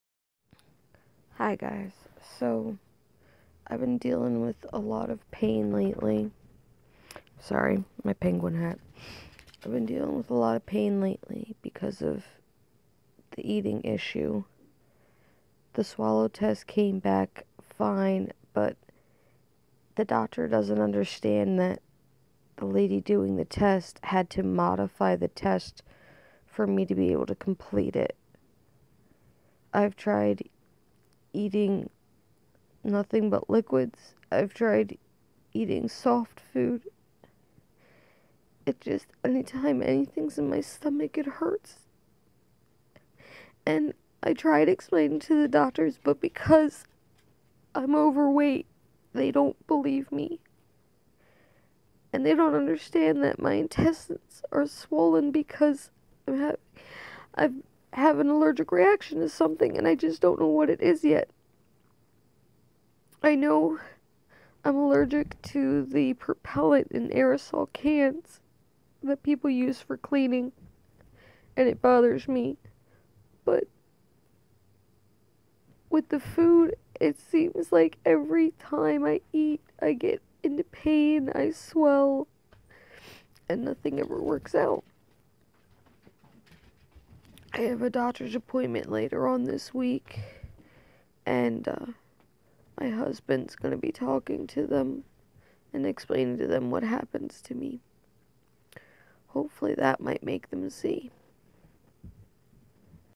Upset and crying
Everytime I eat my stomach hurts so bad and the doctors don't want to do anything yet because of my weight